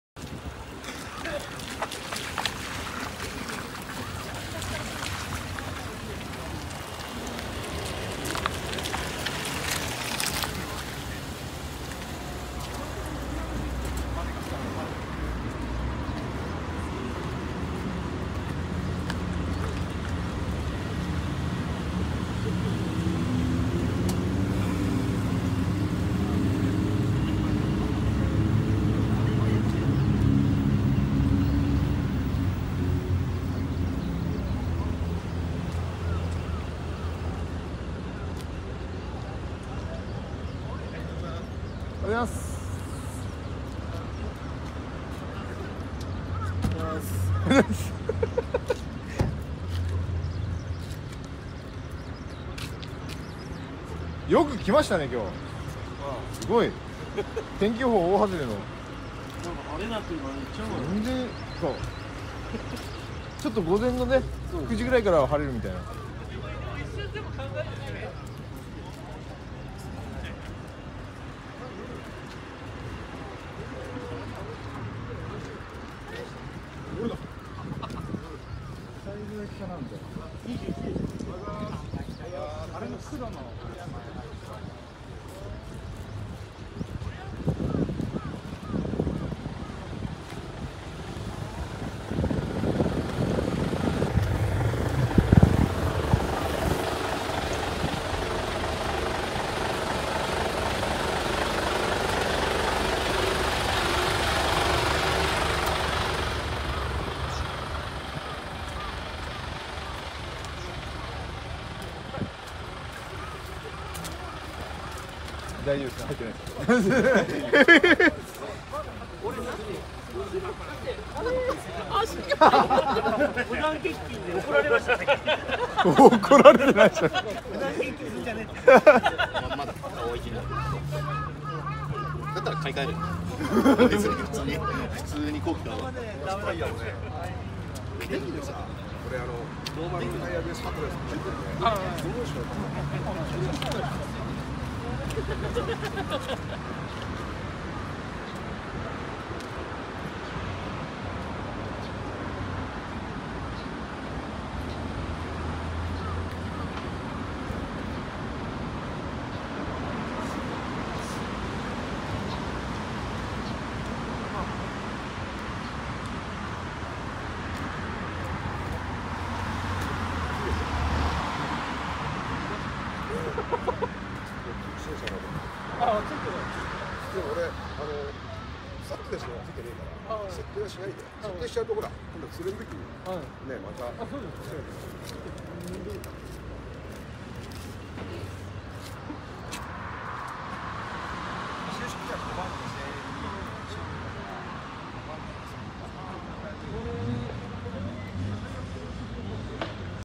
【加藤さんのレクサスLS 600hl や 50センチュリーも到着‼️】セルシオ LS クラウン センチュリー【道の駅 花園MT 2019.3.31 】
いつもご参加いただきありがとうございます！
大黒PAでもお会いできるのかしら笑笑